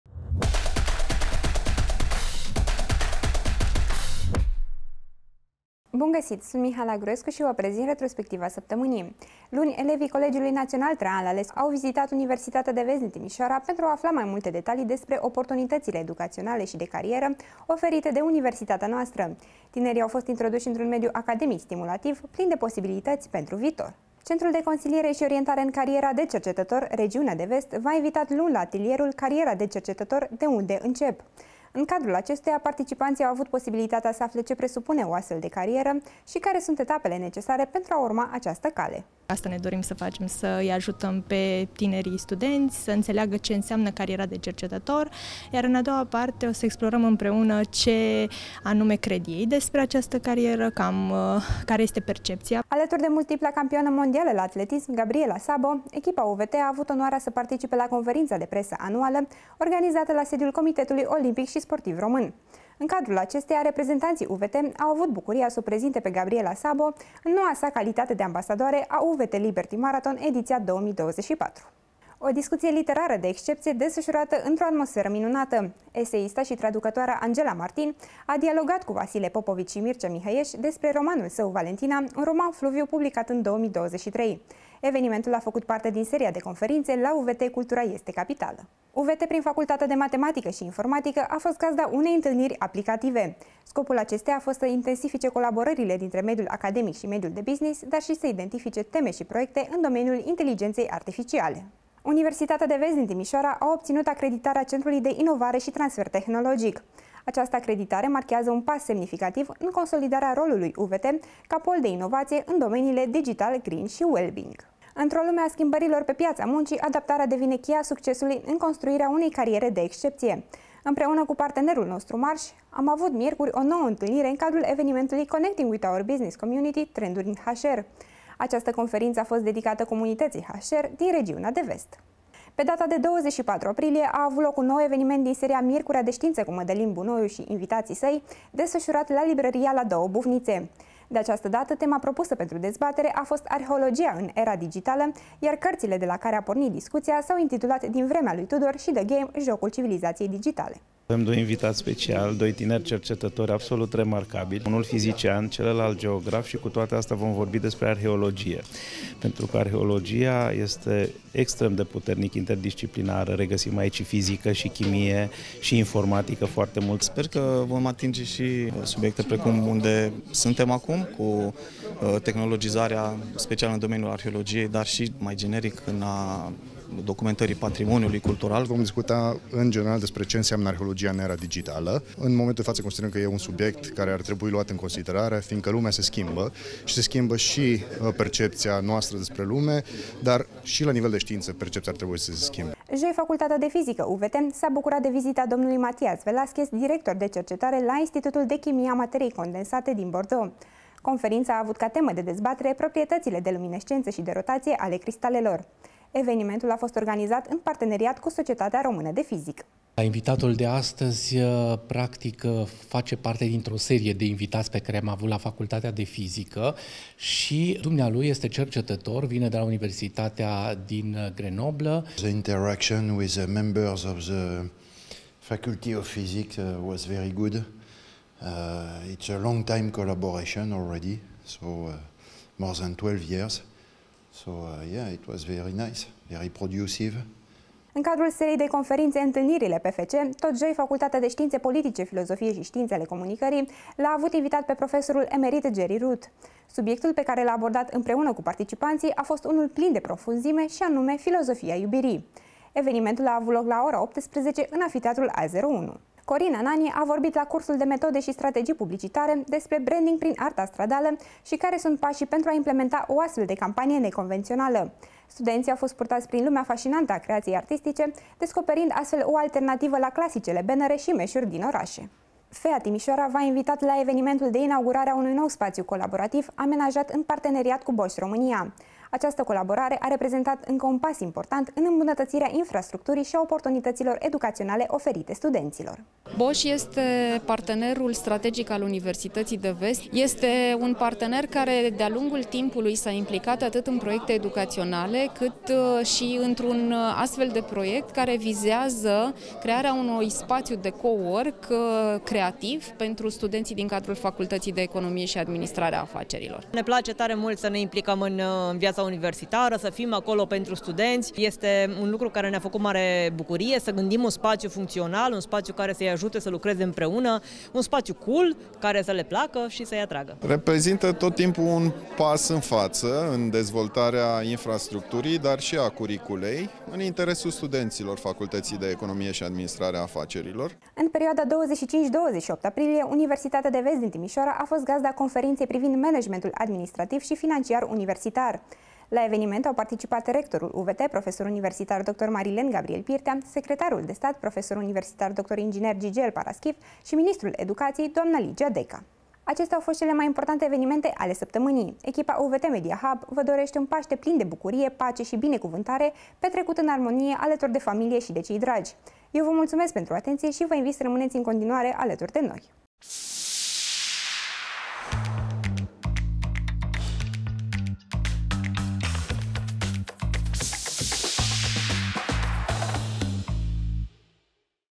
Retrospectiva săptămânii - 29 aprilie 2024
✅Vizita elevilor de la Colegiul Național „Traian Lalescu” din Reșița, atelierul „Cariera de Cercetător - De unde încep?”, Gabriela Szabó - Ambasadoarea UVT Liberty Marathon 2024, conferința „Romanul Valentina și Mitteleuropa de lângă noi”, acreditarea Centrului de Inovare și Transfer Tehnologic, un nou eveniment din seria „Miercurea de știință cu Mădălin Bunoiu și invitații săi”, evenimentul „Connecting with our Business Community. Trend in HR”, evenimentul de inaugurare a unui nou spațiu colaborativ și conferința privind managementul administrativ și financiar universitar.

👉Acestea sunt doar câteva dintre noutățile din această săptămână. Pentru a fi la curent cu ultimele evenimente ce se desfășoară la Universitatea de Vest din Timișoara, vă invităm să urmăriți „Retrospectiva Săptămânii”!